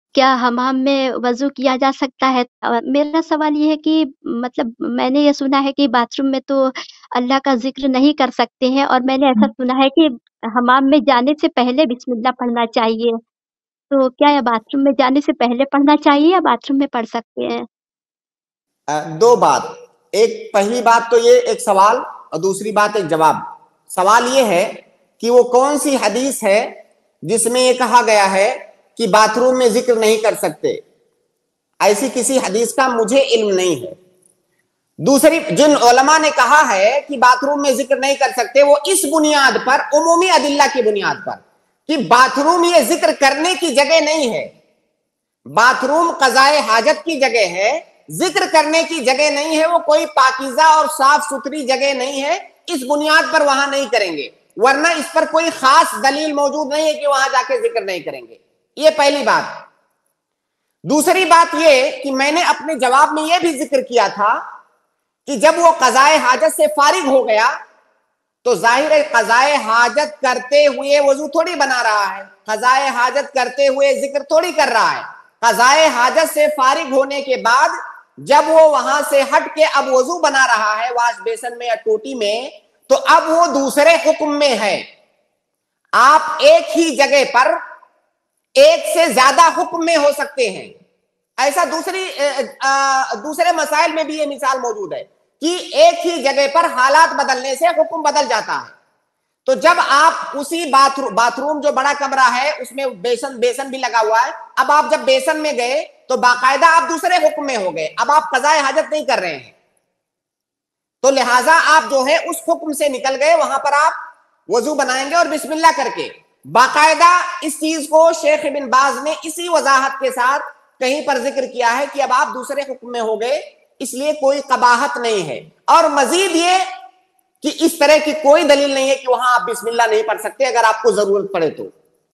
Kya Bathroom Mein Wuzu Karne Ke Liye Bismillah Padh Sakte Hain Ya Nahi? by Dr. Mohammad Naseem
Kya Bathroom Mein Wuzu Karne Ke Liye Bismillah Padh Sakte Hain Ya Nahi? by Dr. Mohammad Naseem Madani iPlus TV Deeni Q&A

UPI : donateilm@icici